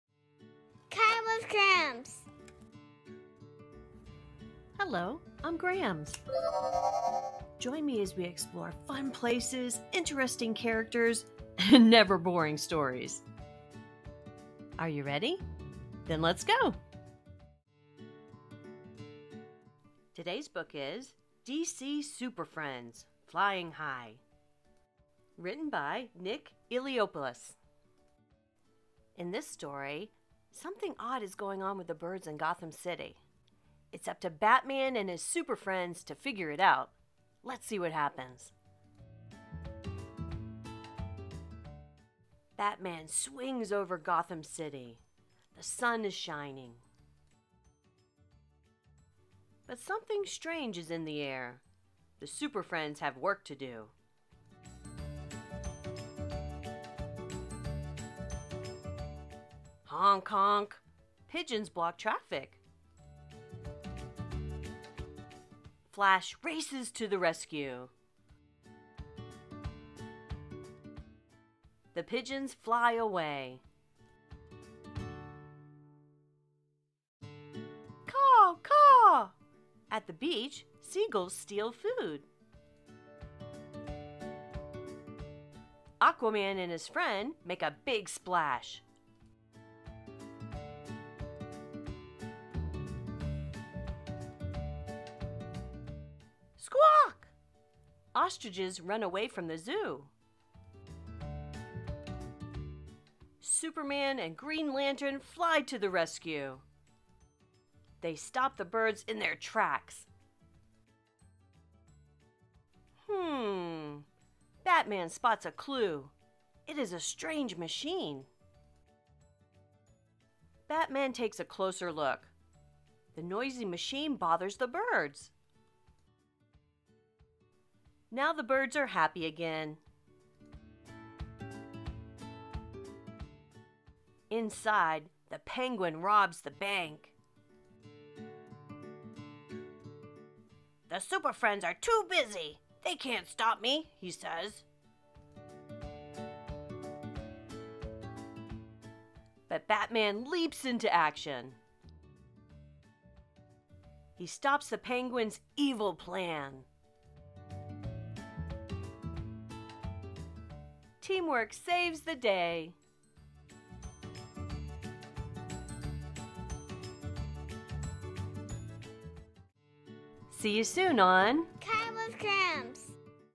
Read along | DC Super Friends, Flying High (with Highlighted words)
Something is wrong with the birds in Gotham City.  It's up to Batman and his Super Friends to figure it out.  Will they be able to bring order back to Gotham City?  Let's see what happens!

Written by Nick Eliopulos
Illustrated by Loston Wallace and David Tanguay

Learning bonus!  Follow along as the words are shown on the screen to build reading skills.

If you like this video,  please consider supporting my work and buy me a cup of coffee